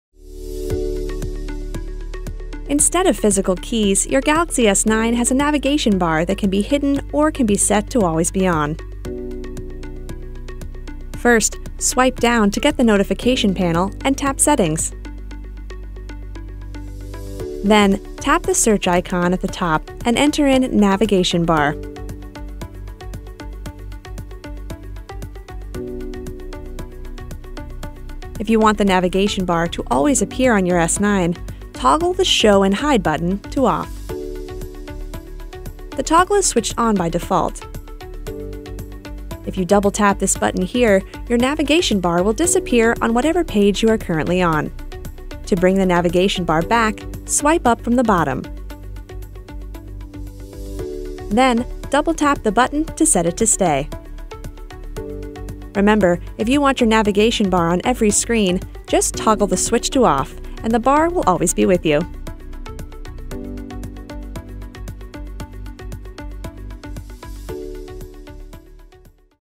How to Show/Hide the Navigation Bar on Samsung Galaxy S9/S9+
The navigation bar at the bottom of your Galaxy S9 can be hidden on certain apps or all the time. You may find yourself having trouble with the bar disappearing when you don't want it to. If so, watch this video to find out how to customize your navigation bar.

We are always open for your comments, questions and requests.